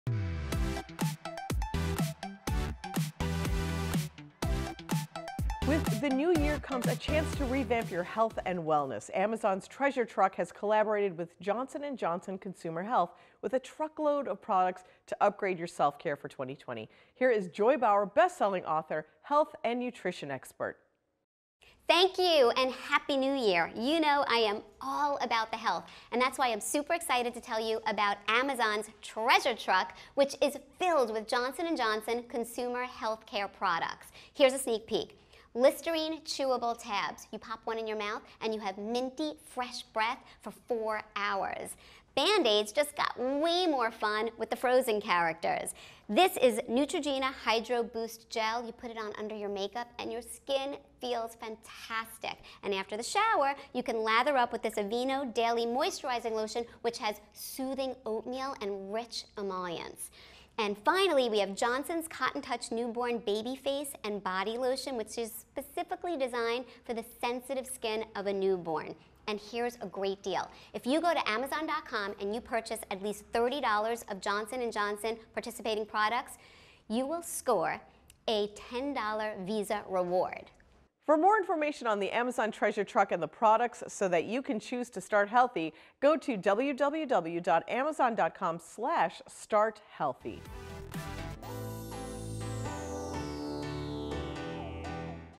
The Amazon Treasure Truck Is Here
To make sure that you have your best year yet, Amazon’s Treasure Truck parked its “New Year New You Health Tour” in Manhattan’s Chelsea neighborhood with a truckload of new gear, products, smoothies, and supplements plus guests could jump in on a workout class from top NYC fitness experts!